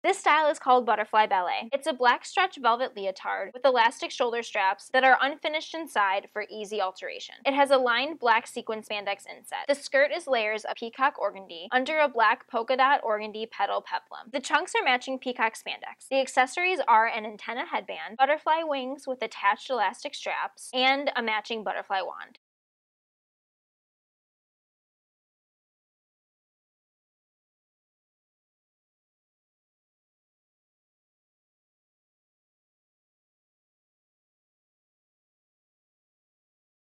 Butterfly Ballet | RC21597 | Revolution Costume Collection
Stretch velvet leotard with spandex trunks has a lined sequin spandex inset and elasticized shoulder straps. Skirt has layers of organdy under an organdy petal peplum. Includes headband, wings, wand, hanger, and garment bag.

Stretch velvet leotard with spandex trunks has a lined sequin spandex inset and elasticized shoulder straps. Skirt has layers of organdy under an organdy petal peplum.

Includes headband, wings, wand, hanger, and garment bag.